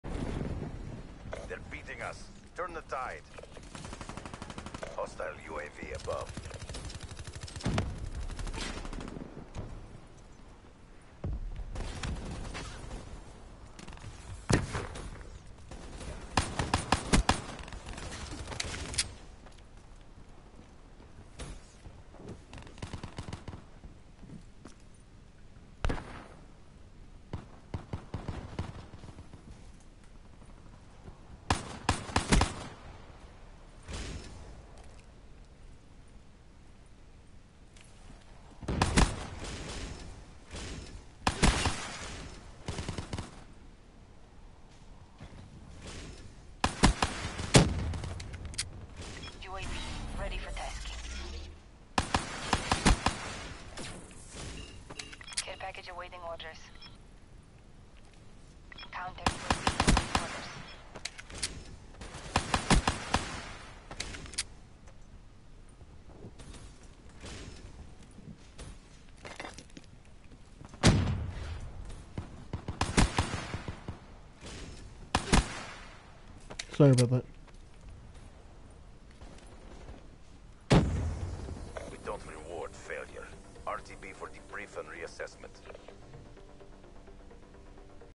MR6 9-0 Hardcore Team Death Match Play
Sickening MR6 Play
 9x One Shot One Kill
 1x HeadShot
 3x Double Kill
 3x Kingslayer
1x Revenge
1x Grounded